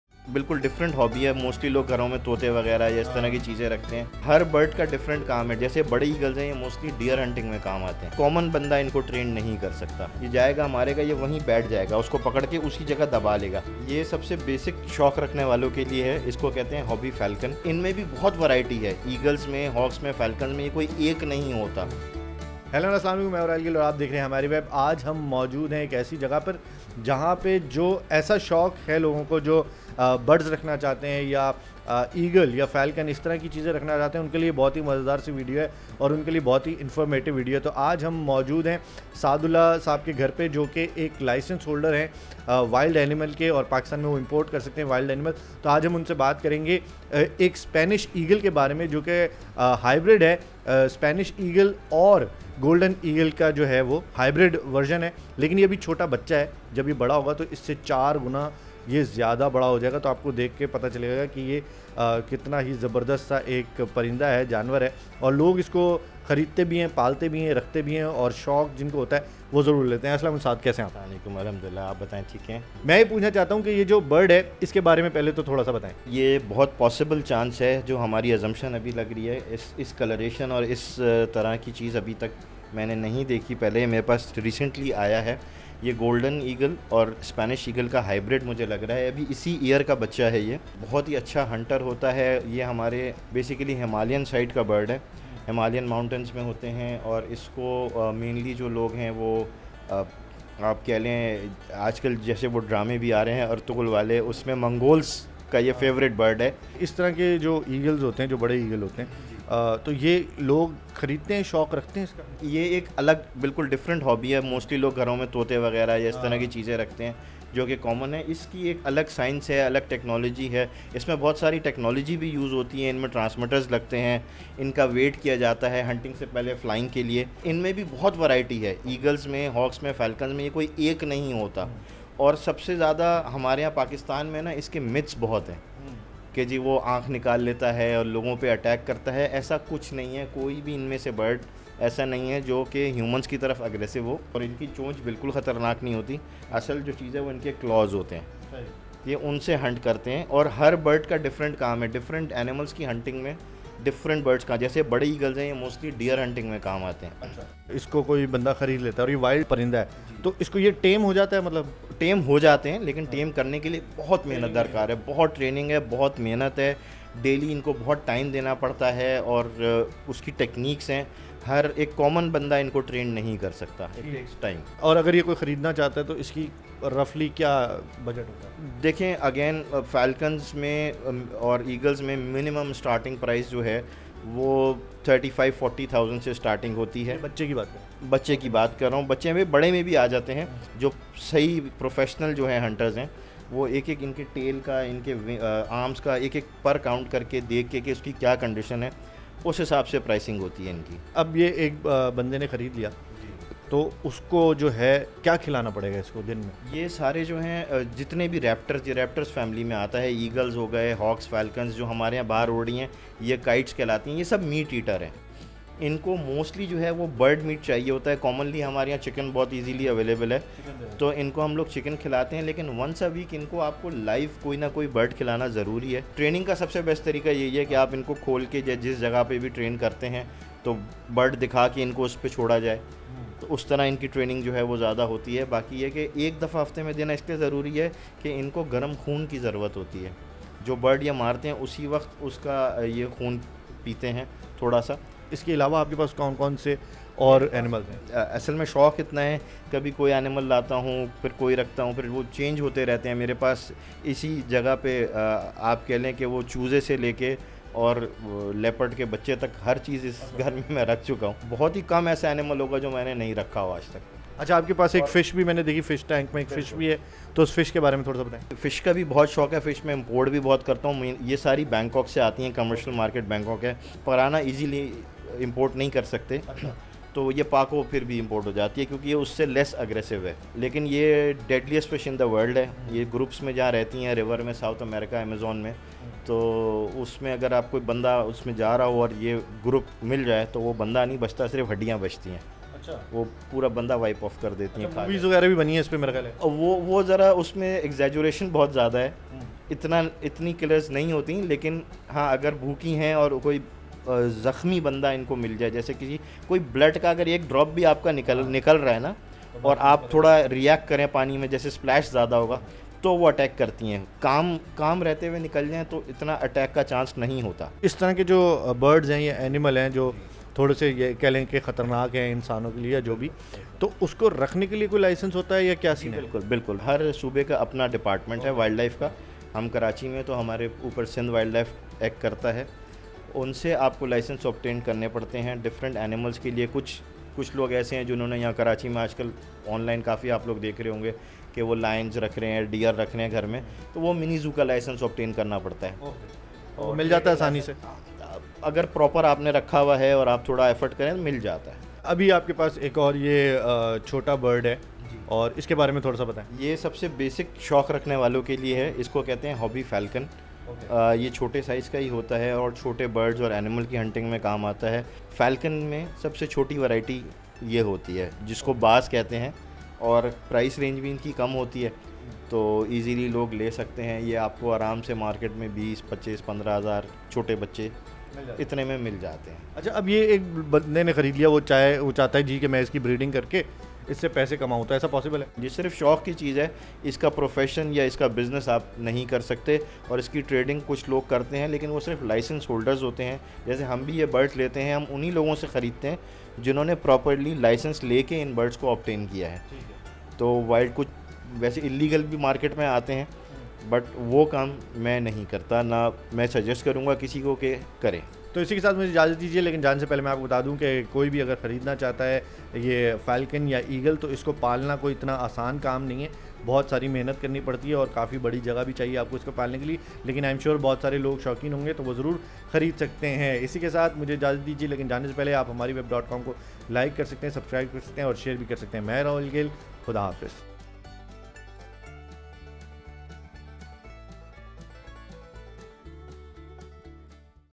Imported Birds & Animals | Hybrid Eagles | Eagles Price in Pakistan | Falcon
The relation between animals and humans is much old. From the beginning, humans are keeping birds and animals as pets. However, in Pakistan, the business of buying and selling precious birds is also gaining popularity. People not just Eagles and Falcons as a pet but also utilize them for deer hunting. However, there is a huge variety among the Falcons, Eagles, and other birds. Professional training is required for birds that do hunting. In this video, you can see the hybrid breed between Spanish Eagle and Golden Eagle and can find out some crucial information. Just check out the video for further details.